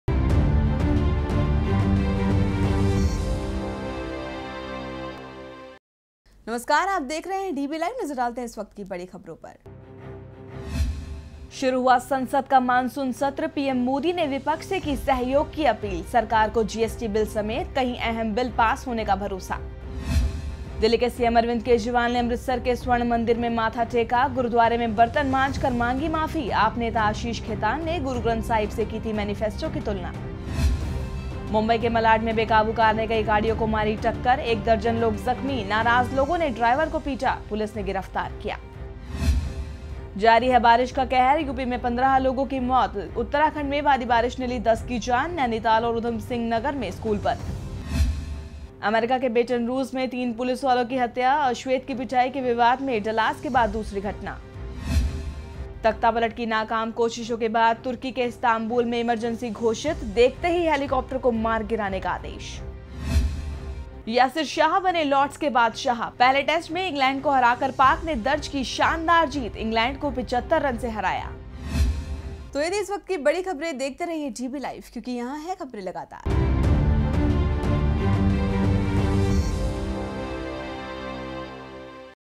DBLIVE | 18 JULY 2016 | News Headlines
1...शुरू हुआ संसद का मानसून सत्र... पीएम मोदी ने विपक्ष से की सहयोग की अपील...सरकार को जीएसटी बिल समेत कई अहम बिल पास होने का भरोसा।
2..दिल्ली के सीएम अरविंद केजरीवाल ने अमृतसर के स्वर्ण मंदिर में मत्था टेका...गुरूद्वारे में बर्तन मांझकर मांगी माफी...आप नेता आशीष खेतान ने गुरू ग्रंथ साहिब से की थी मेनिफेस्टो की तुलना।
3..मुंबई के मलाड में बेकाबू कार ने कई गाड़ियों को मारी टक्कर... 1 दर्जन लोग ज़ख्मी...नाराज़ लोगों ने ड्राइवर को पीटा...पुलिस ने गिरफ्तार किया।
4..जारी है बारिश का कहर...यूपी में 15 लोगों की मौत...उत्तराखंड में भारी बारिश ने ली 10 की जान... नैनीताल और उधमसिंह नगर में स्कूल बंद
5..अमेरिका के बेटन रूज में तीन पुलिस वालों की हत्या...अश्वेत की पिटाई के विवाद में डलास के बाद दूसरी घटना
6..तख्तापलट की नाकाम कोशिशों के बाद तुर्की के इस्तांबुल में इमरजेंसी घोषित...देखते ही हेलिकॉप्टर को मार गिराने का आदेश
7..यासिर शाह बने लॉर्ड्स के बादशाह...पहले टेस्ट में इंग्लैंड को हराकर पाक ने दर्ज की शानदार जीत... इंग्लैंड को 75 रन से हराया